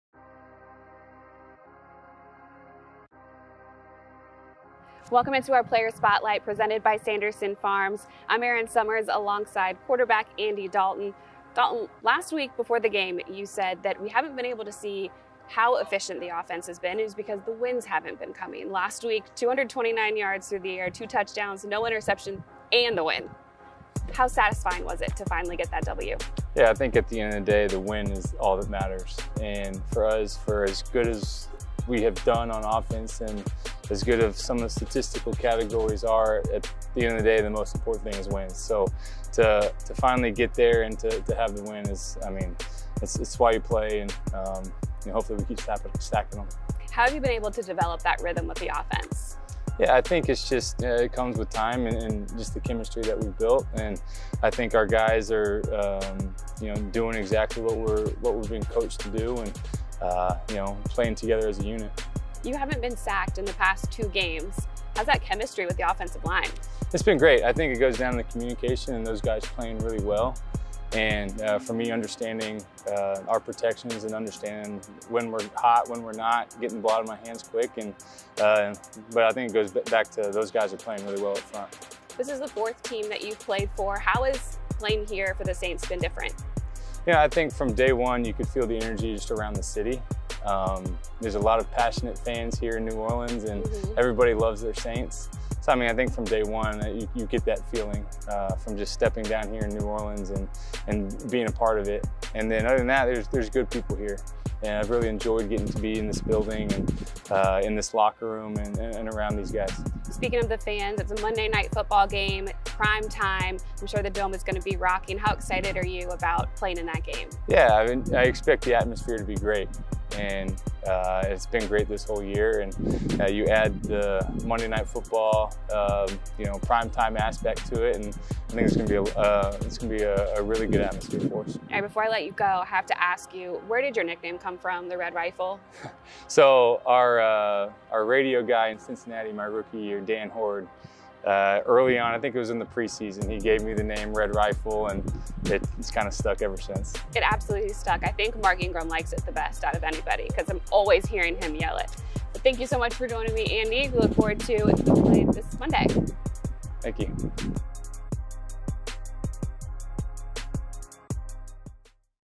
Andy Dalton | 2022 Saints Player Spotlight
New Orleans Saints quarterback Andy Dalton sits down with team reporter Erin Summers ahead of Monday Night Football in Week 9 against the Baltimore Ravens on the Saints Player Spotlight presented by Sanderson Farms.